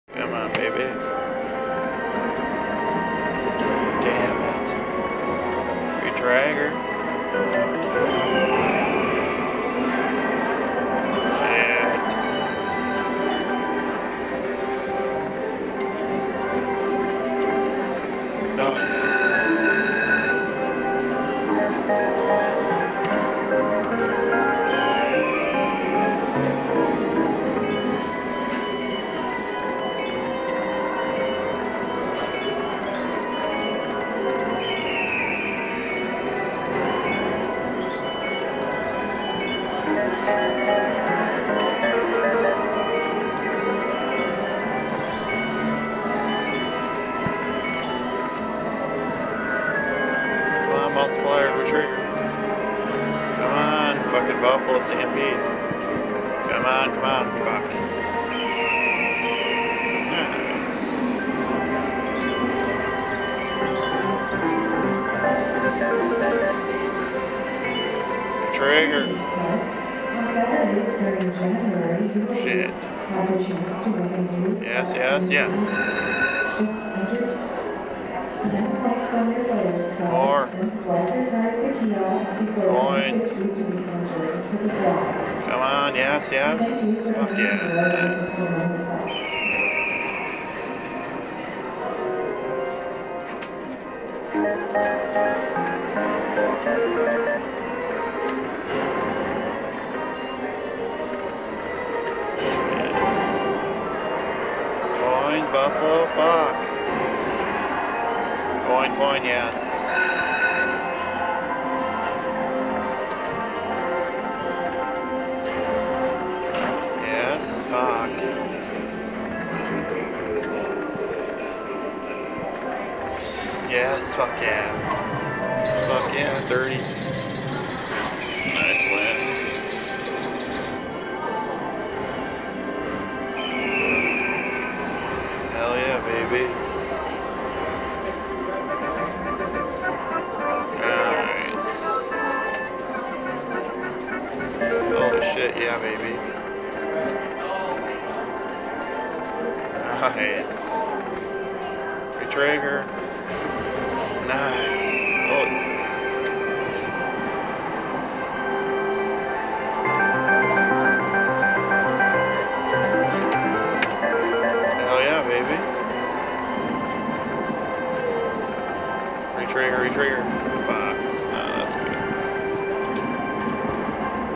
BIG WIN KENNY STYLE!! Aristocrat Buffalo Original Max Multiplier
Dedicating this to a friend Ken who plays this slot machien with less  reels but max multiplier i figured it would try it on the original buffalo and damn it worked pretty good!